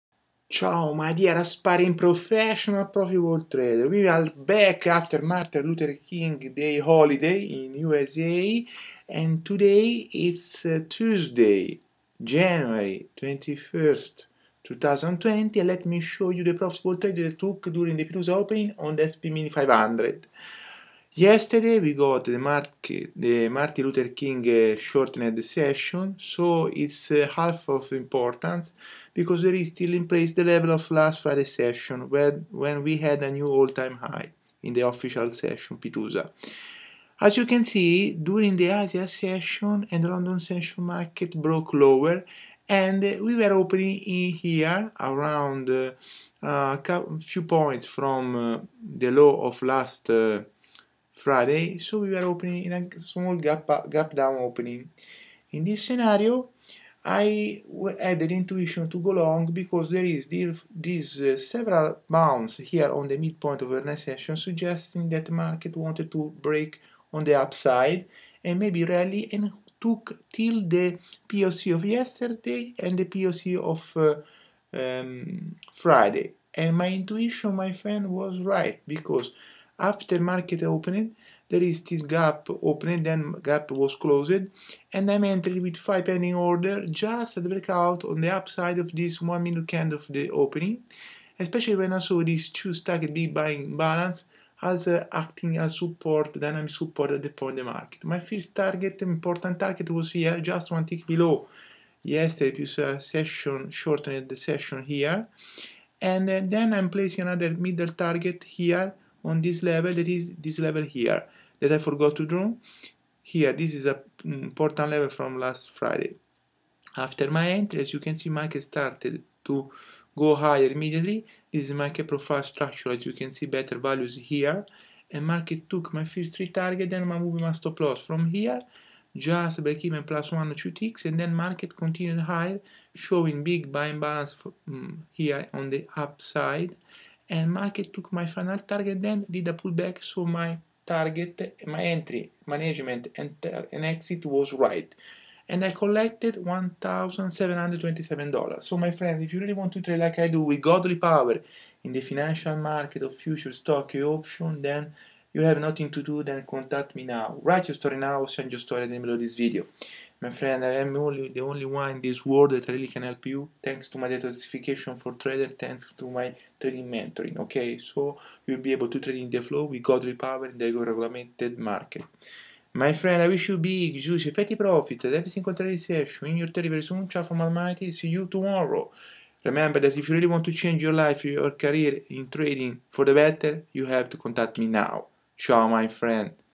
21 Jan 2020 - Long At The Small Gap Down Pit USA Open Toward Yesterday's POC- Market Profile Trading
21 January 2020 - Long At The Small Gap Down Pit USA Opening Toward Yesterday's POC - Market Profile Trading - Trading Futures Stocks & Options Detoxified And With Godly Powers